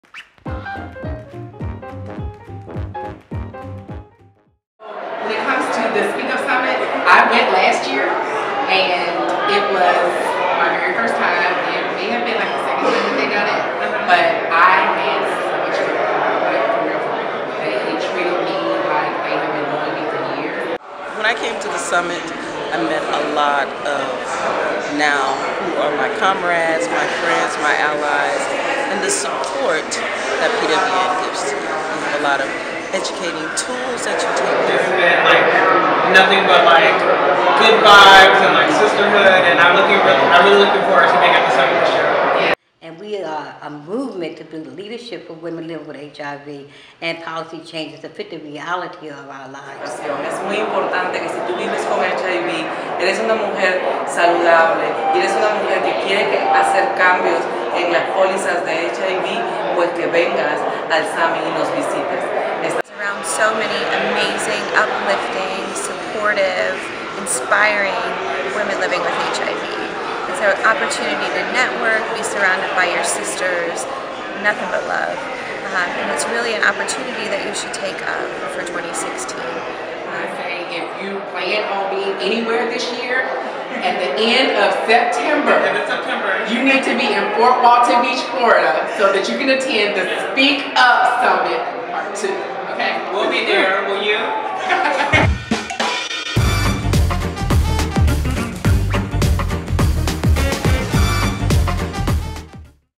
Everyone's Talking About Speak Up! 2016
Everyone is talking about SPEAK UP! 2016, the only National Leadership Summit for women living with HIV, hosted by Positive Women's Network - USA, in Fort Walton Beach, FL, Sept. 27-30! Listen to why SPEAK UP! 2016 is THE event of the year--or a lifetime--for women living with HIV of all ages and backgrounds!